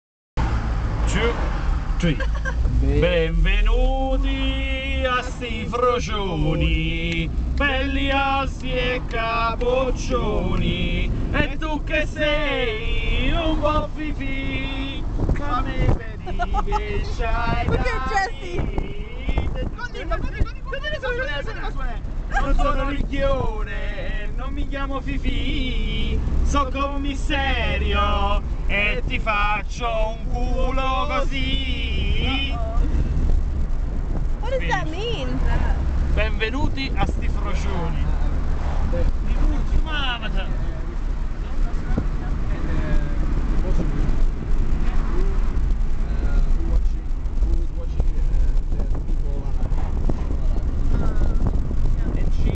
Singing in the Panda
In Fulvio's trusty Fiat, we crused down the Italian Alps singing Italian songs.